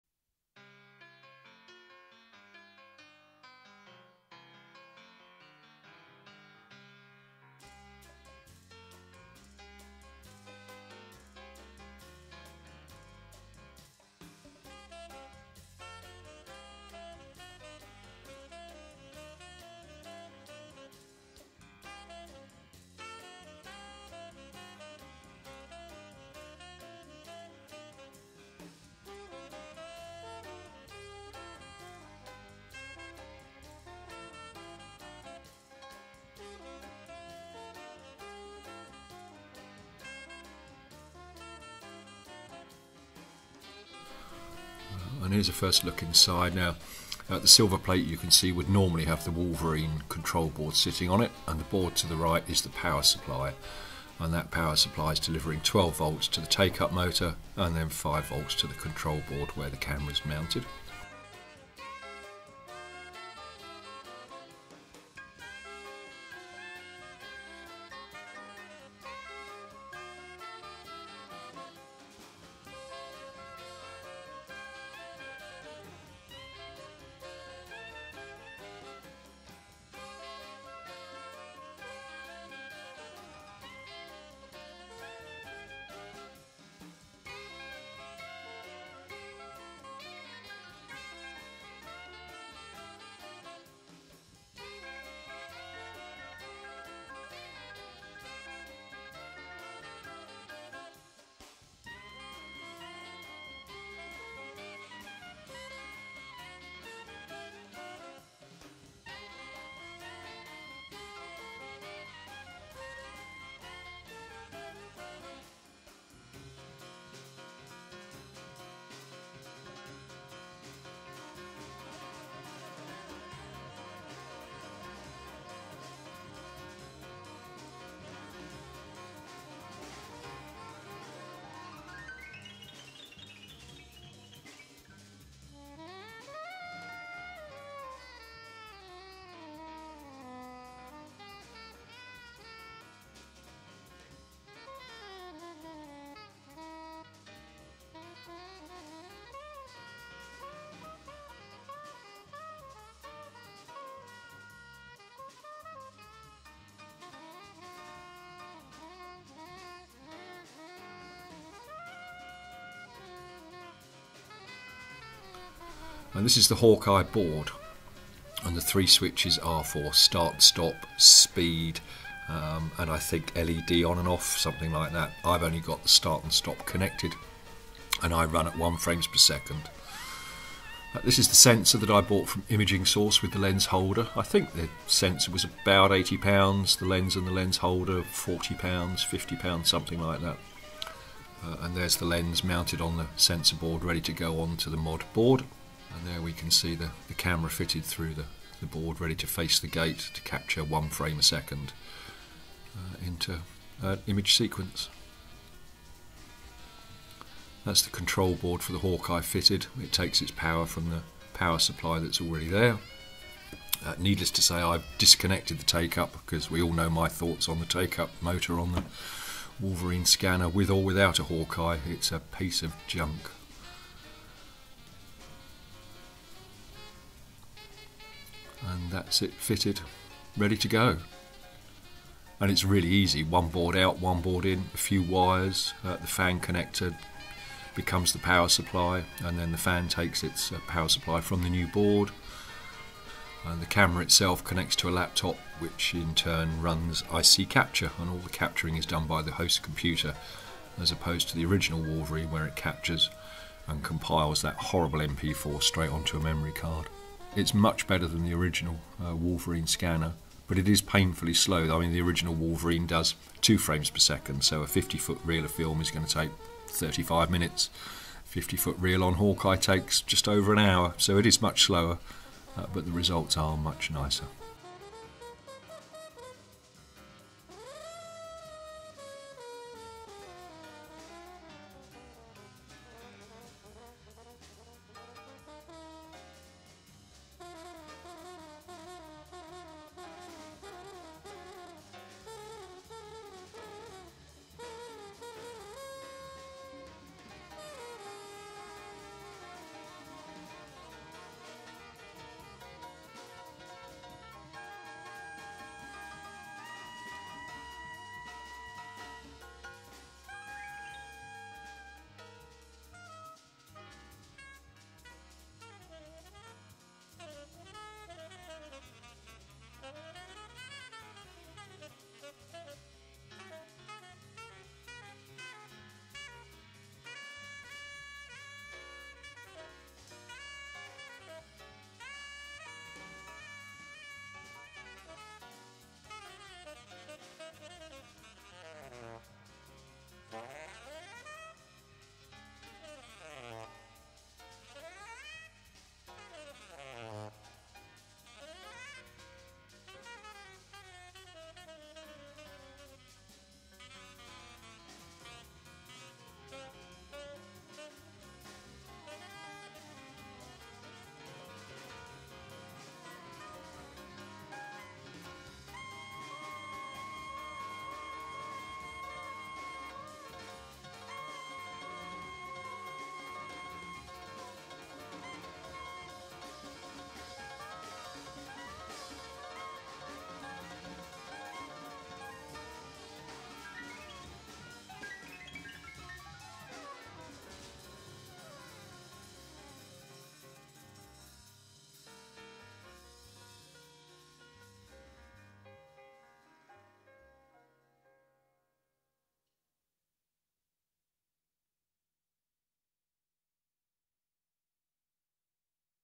WOLVERINE CINE SCANNER / HAWKEYE MOD
I was asked for more detail on the Wolverine cine scanner and the modification that mine has.

This is a quick look at the mod, when I find the time I will do a teardown video so you can see what it was and what is is today.

This is just a quick look so you can see what it's about.

The music track in this edit is purposely very quiet so don't crank your volume up thinking something is wrong with your computer.

The camera is around £100 plus lens and lens holder at around £60

I would not suggest buying a basic Wolverine, if you have to have one, get the Pro which takes 7" reels and does a reasonable job of scanning.

You can search YT for the Pro Wolverine, there are examples of it's scans online.

You can find Stan at the 8mm film forum, a quick google will bring that to the top of the results, look in the 'film to digital section'

Be aware this website demands you have your real name and real face photo, I have a problem with that so I am not a member any longer but if you find the archive forum on the wolverine scanner, you will find the input I had during the early development of Stan's Modification.